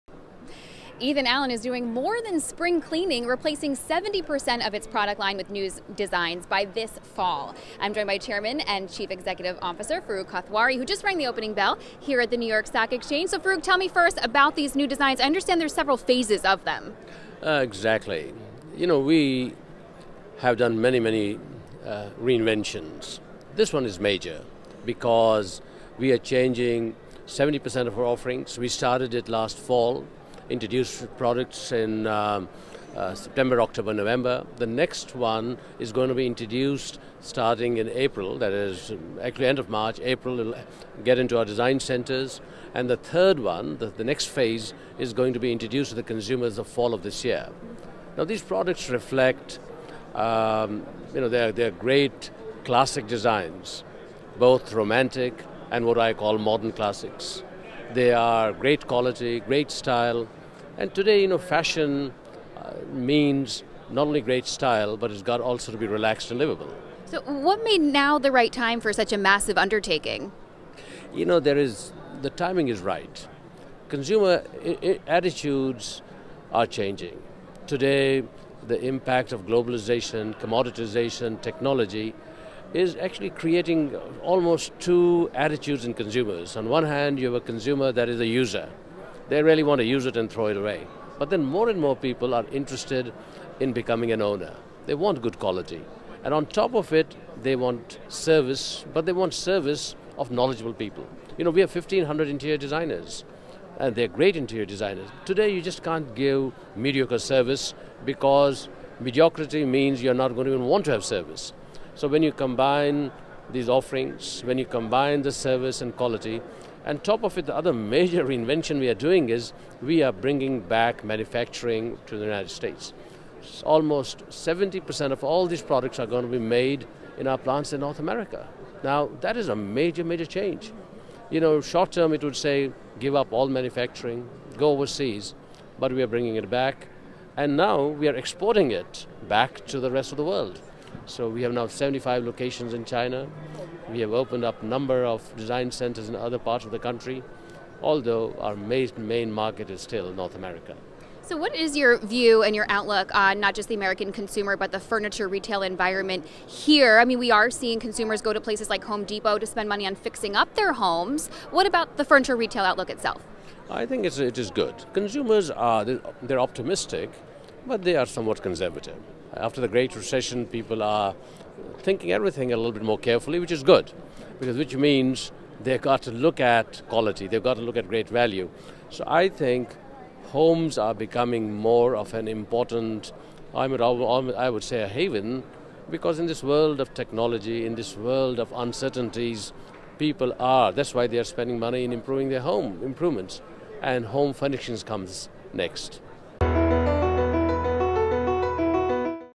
CEO of Furniture Chain Ethan Allen Predicts Home Furnishing After Improvement Trend
Ethan Allen Interiors chairman and chief executive officer Farooq Kathwari reveals the company's new initiatives to completely replace 70% of its product line with new designs by fall 2015. Kathwari says now is the right time for such a large undertaking because consumer attitudes are changing due to factors like globalization and trends in technology. Kathwari says more and more consumers want to be owners of good quality products sold to them with great service. He reveals how Ethan Allen is trying to appeal to those consumers, which he believes are optimistic right now, but still conservative. As consumers have been spending money at retailers like Home Depot to improve their home, Kathwari says furnishing their homes is next.